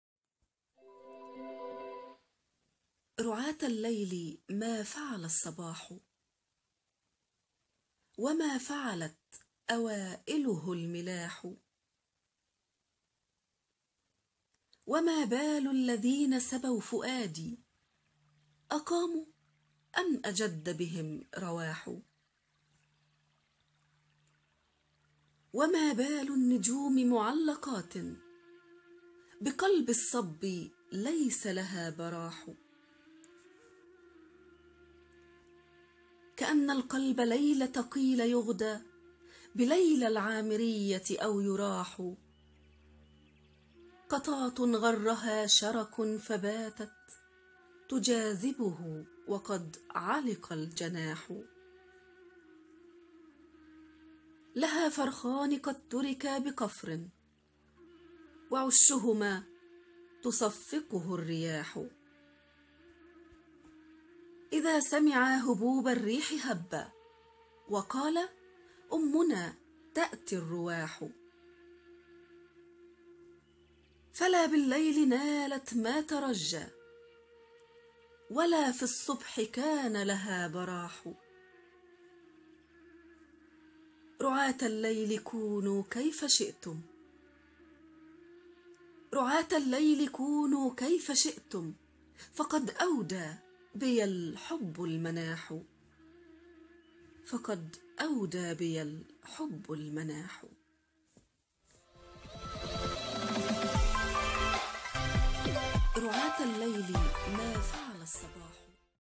الشاعر « قيس بن الملوح » في قصيدته : ( رُعَاة الليل ) إلقاء سوبر سها
الشاعر «   قيس بن الملوح » في قصيدته :

( رعاة الليل ما فعل الصباح)

بصوت سها

رُعاةَ اللَيلِ ما فَعَلَ الصَباحُ

وَما فَعَلَت أَوائِلُهُ المِلاحُ

وَما بالُ الَّذينَ سَبوا فُؤادي

أَقاموا أَم أَجَدَّ بِهِم رَواحُ

وَما بالُ النُجومِ مُعَلَّقاتٌ

بِقَلبِ الصَبِّ لَيسَ لَها بَراحُ

كَأَنَّ القَلبَ لَيلَةَ قيلَ يُغدى

بِلَيلى العامِرِيَّةِ أَو يُراحُ

قَطاةٌ عَزَّها شَرَكٌ فَباتَت

تُجاذِبُهُ وَقَد عَلِقَ الجَناحُ

لَها فَرخانِ قَد تُرِكا بِقَفرٍ

وَعُشُّهُما تُصَفِّقُهُ الرِياحُ

إِذا سَمِعا هُبوبَ الريحِ هَبّا

وَقالا أُمَّنا تَأتي الرَواحُ

فَلا بِلَّيلِ نالَت ما تُرَجّي

وَلا في الصُبحِ كانَ لَها بَراحُ

رُعاةَ اللَيلِ كونوا كَيفَ شِئتُم

فَقَد أَودى بِيَ الحُبُّ المُتاحُ